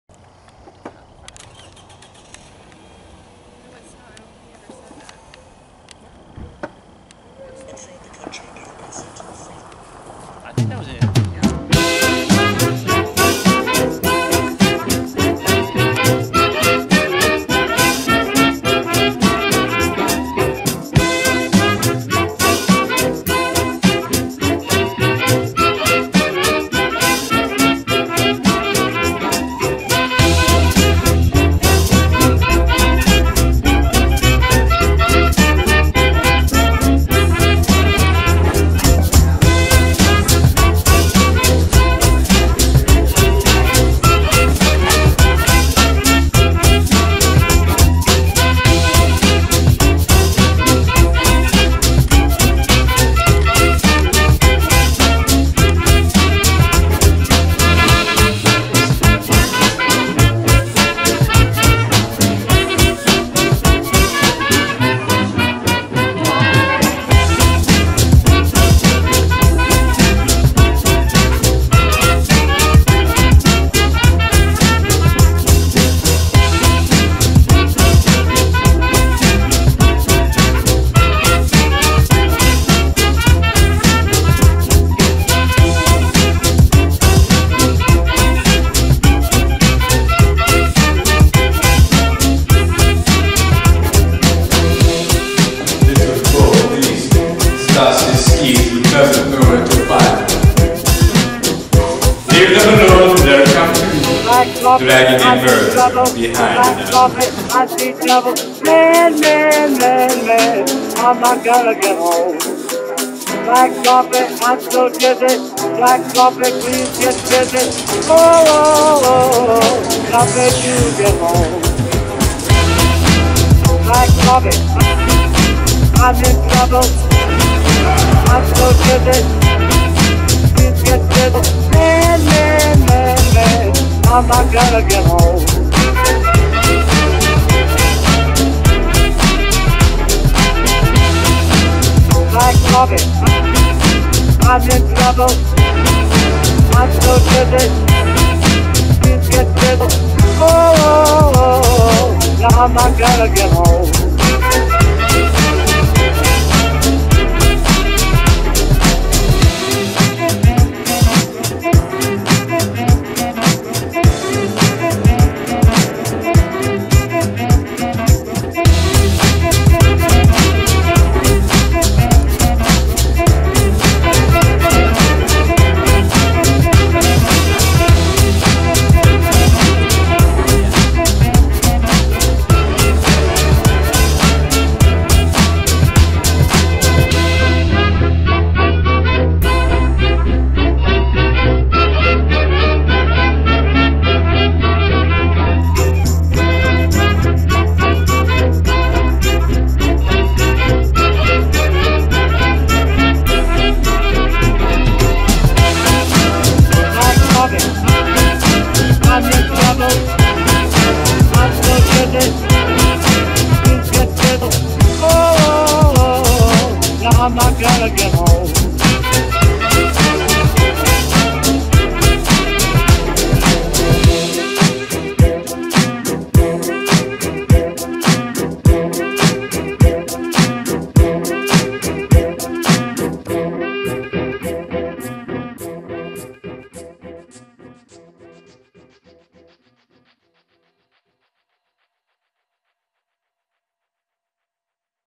The Mousetrap: Behind the Scenes
So originally my next intended published video was going to be of an airshow I recently went to. However due to a lack of decent quality footage that project has been scrapped indefinitely. Instead we have one final video about the play I was in recently in. You saw the promo now go behind the scenes. Enjoy